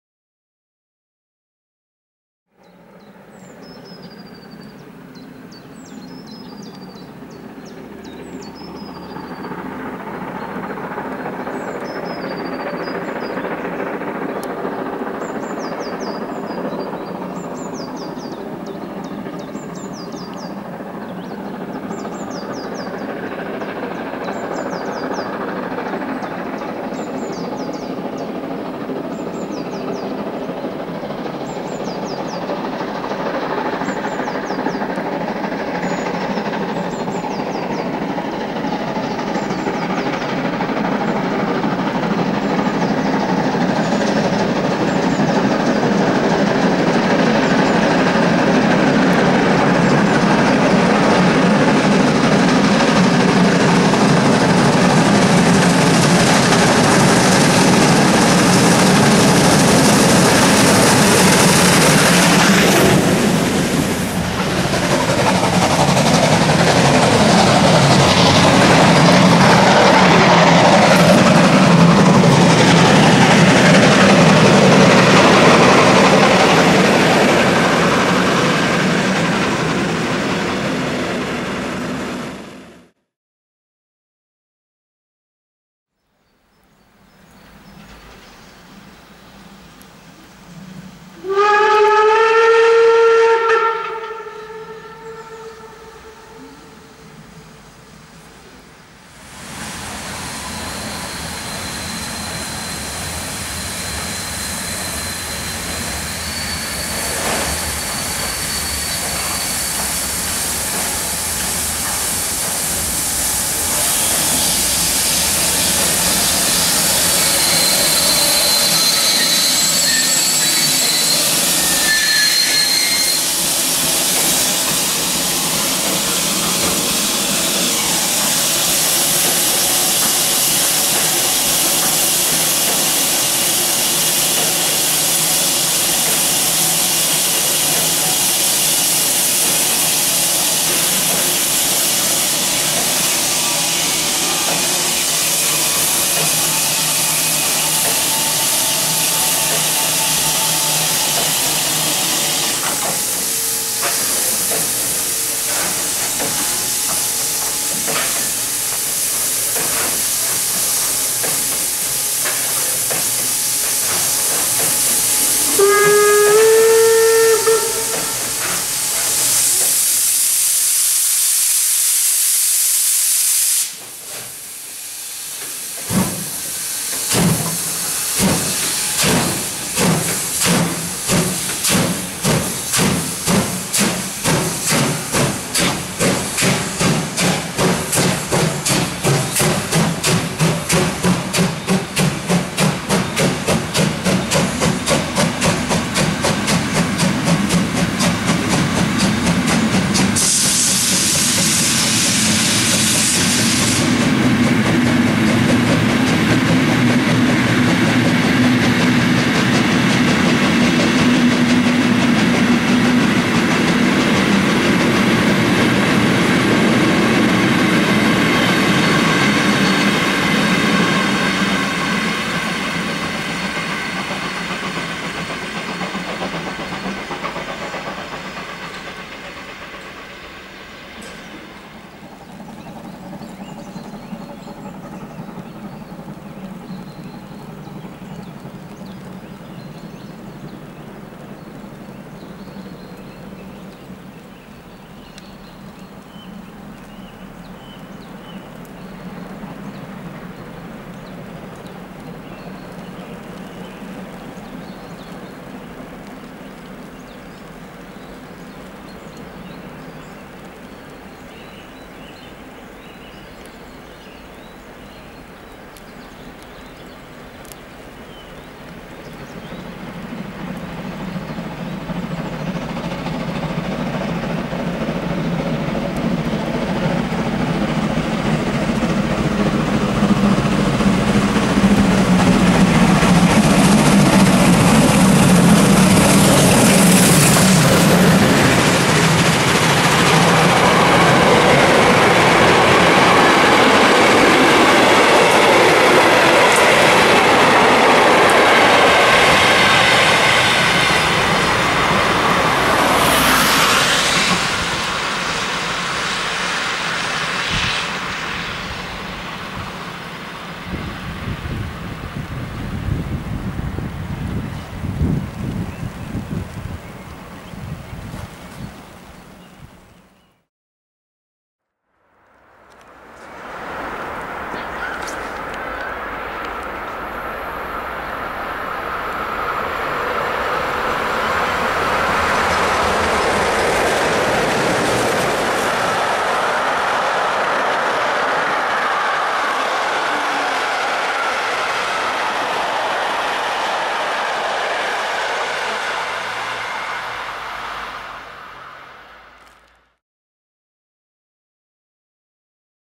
Die "Altmeister" mit 52 8079-7 am Klotzscher Berg, wieder da: 50 3648-8...
-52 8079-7 am 26.3.16 am Klotzscher Berg (letzte Fahrt der beiden"Altmeister")
-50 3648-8 am 26.3.16 in Pirna und der sächs. Schweiz
-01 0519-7 und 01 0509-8 am 2.4.2016 in Dresden